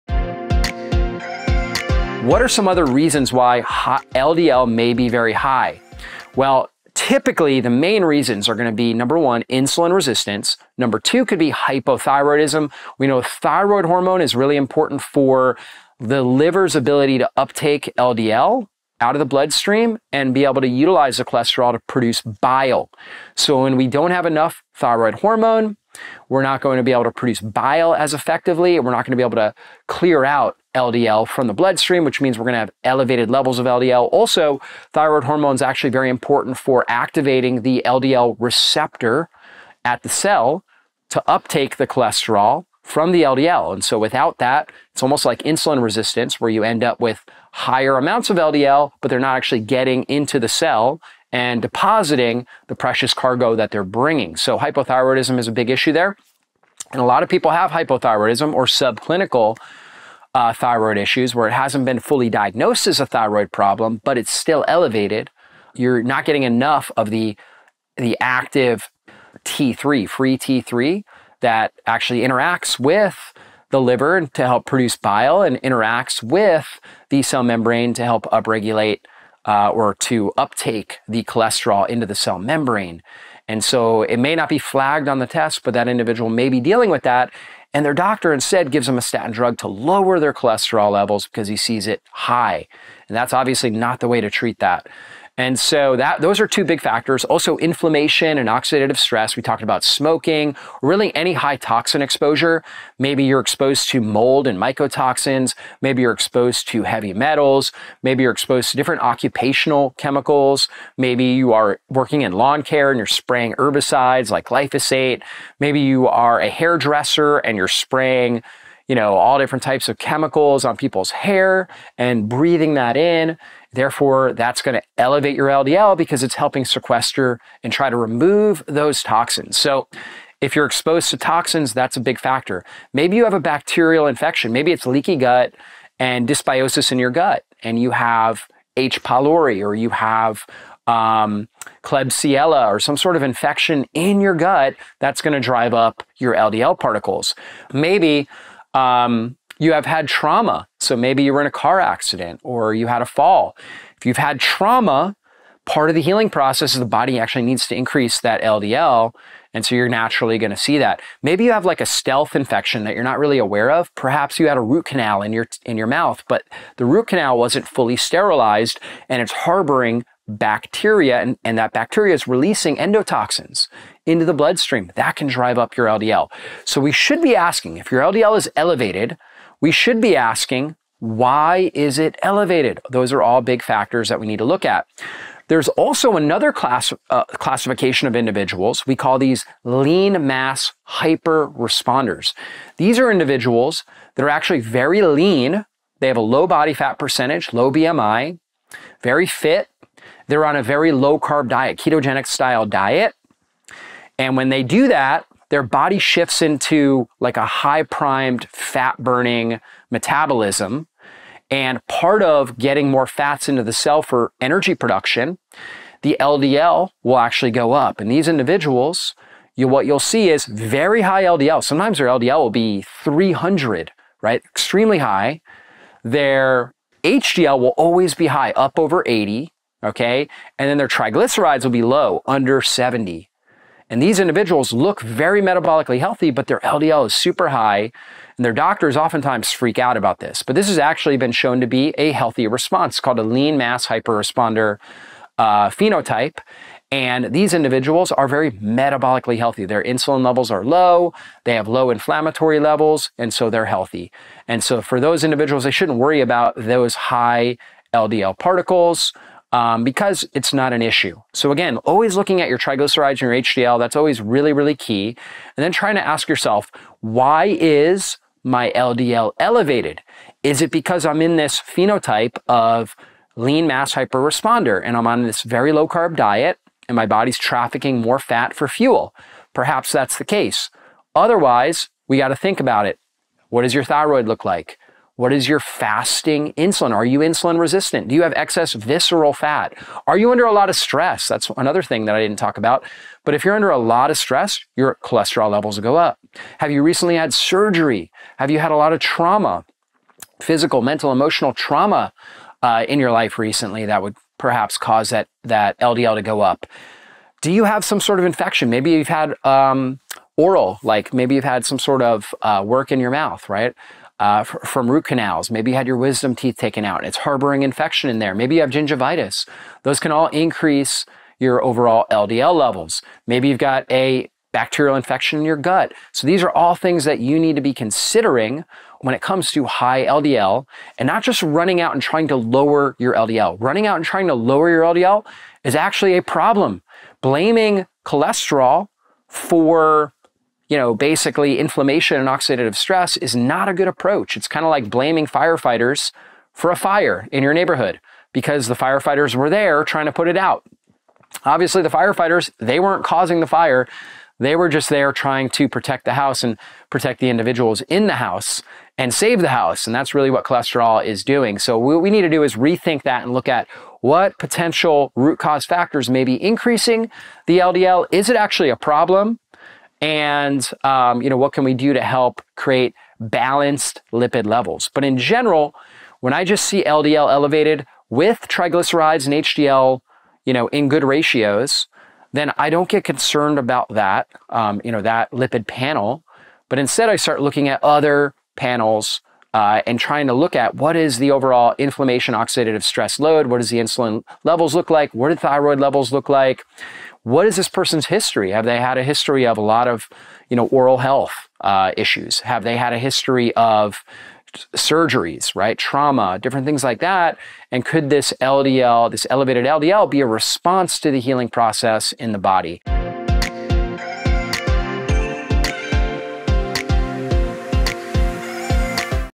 Is High LDL Cholesterol Always Bad? The Truth Revealed!
🚨 In this eye-opening episode, I will uncovers the real reasons behind high LDL (low-density lipoprotein) cholesterol—and why it’s not always the villain it’s made out to be! From insulin resistance and thyroid issues to inflammation, toxins, and even gut infections, we break down the hidden factors affecting your cholesterol levels.

If you’ve ever questioned your cholesterol levels, this is a must-watch for uncovering the root causes and taking control of your health! 💪❤️

0:00 🚨 The Hidden Causes of High LDL Cholesterol
0:34 🦋 Is Your Thyroid Secretly Raising Your Cholesterol?
1:35 🌍 Environmental & Health Triggers You Need to Know
2:40 🩺 Undiagnosed Issues That Could Be Spiking Your LDL
3:05 💪 The Lean Mass Hyper-Responder Mystery Explained
4:21 🔬 Key Factors for a Complete Cholesterol Assessment
5:31 ❌ The Biggest Cholesterol Myth—Debunked!